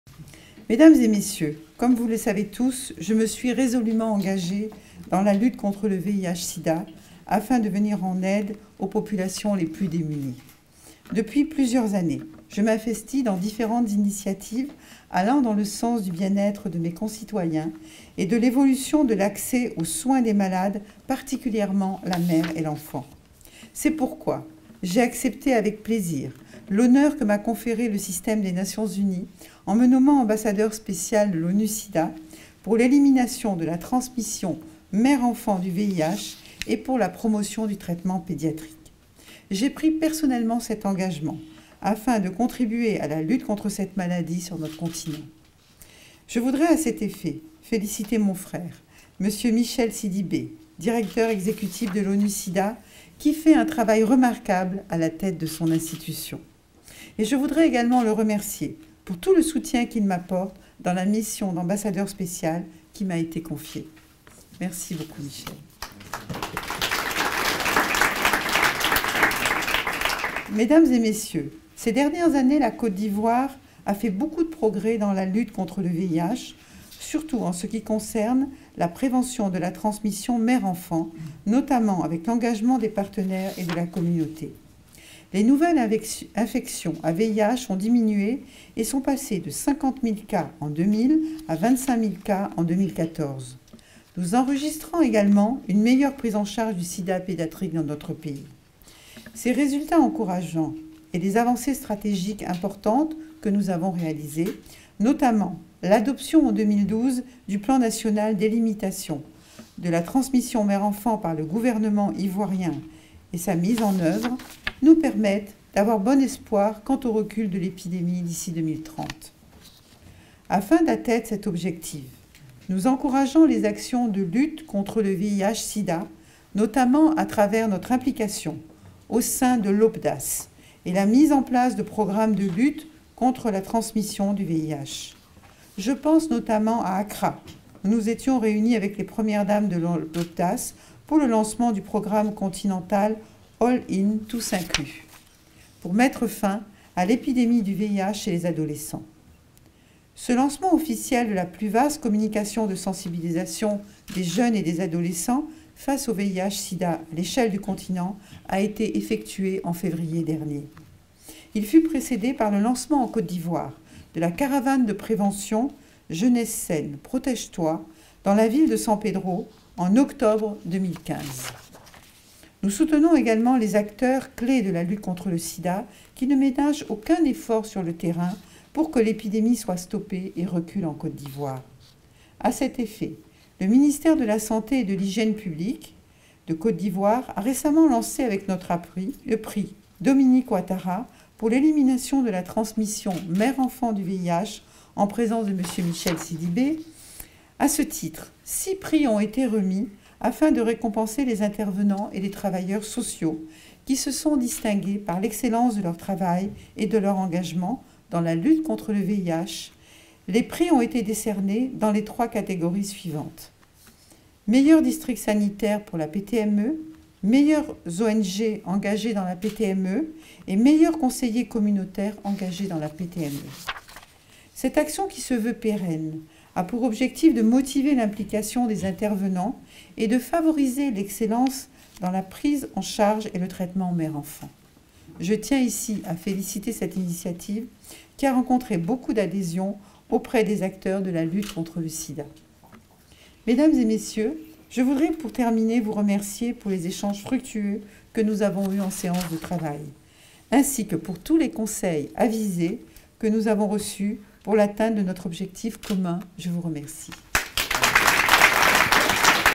Dominique OUATTARA, Première Dame au siège de l'ONUSIDA à Genève le 25 Avril 2016
Dominique Claudine OUATTARA, née NOUVIAN naît le 16 Décembre 1953 à Constantine. Elle est Ivoirienne d'origine Française.
A l'accession de son époux à la magistrature suprême, Madame Dominique OUATTARA renonce à sa brillante carrière de chef d'entreprise pour se mettre exclusivement au service de ses concitoyens.